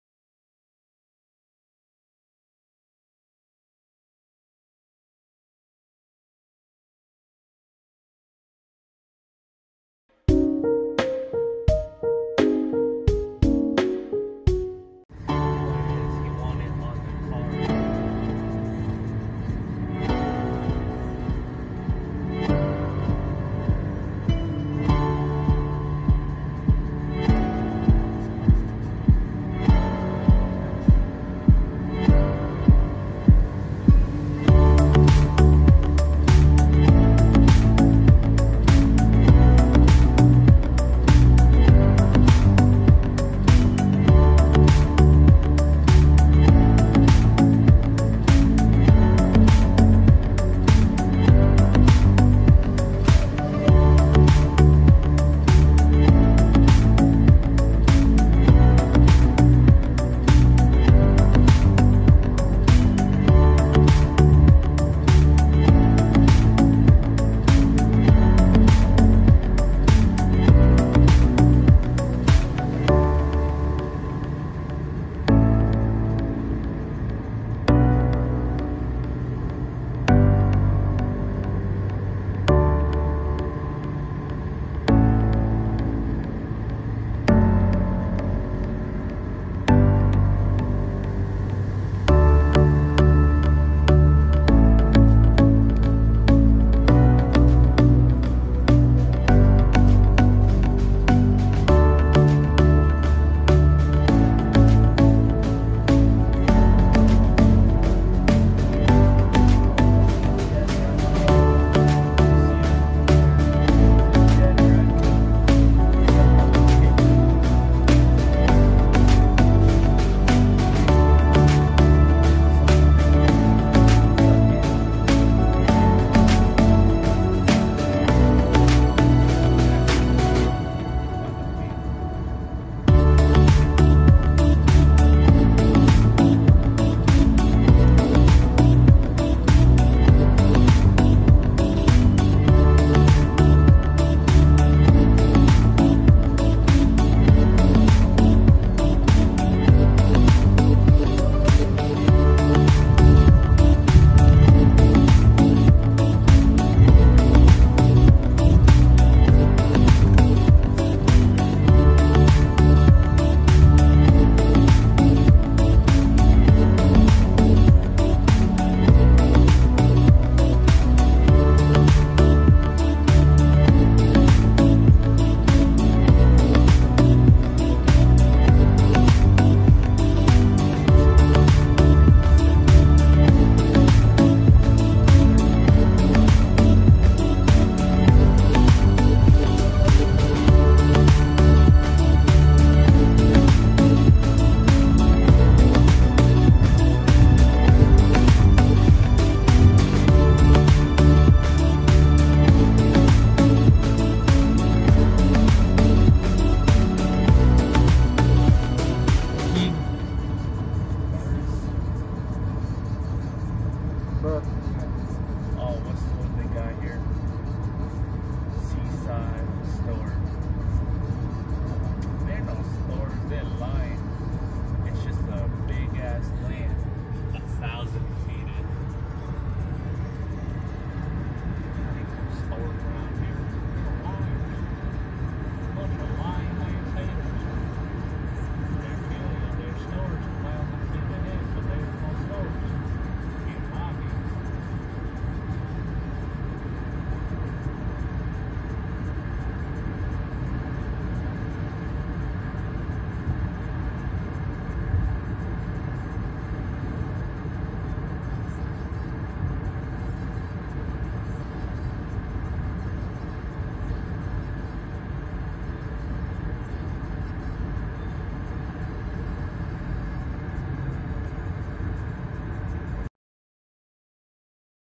Đời sống ở Mỹ, Lái xe từ Virginia về Boston xuyên qua miền quê Route 13 và 113
Thông thường người ta lấy cao tốc Route 95 để đi xuyên qua nhiều tiều bang ở vùng đông bắc Mỷ của 13 tiều bang đầu tiên , nhưng nếu các bang chọn option đi đương miền quê xuyên qua những cánh đổng thì các bạn nên đi  tuyến route13 và route 113 từ bang Virginia về Boston USA